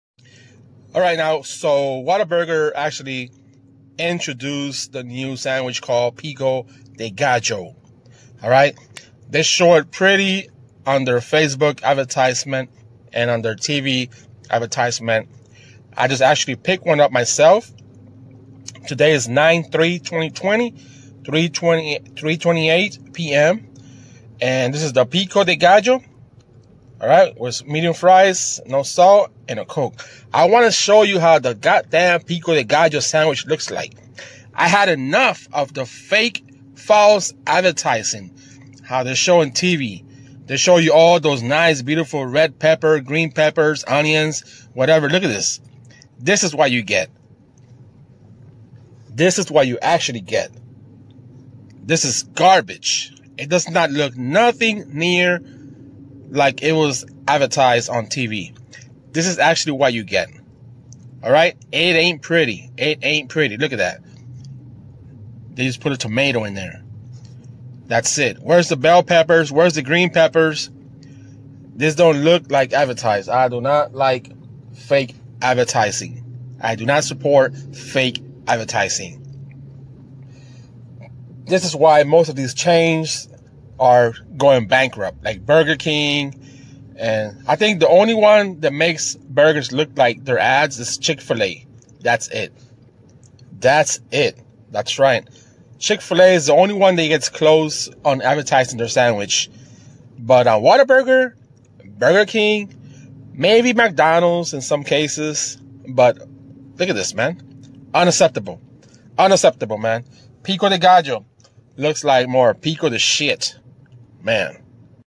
Whataburger Pico de Gallo
So I decided to try the Whataburger Pico de Gallo Burger, I was really, really disappointed looked nothing like the advertising.  It tastes really bad, Here's proof.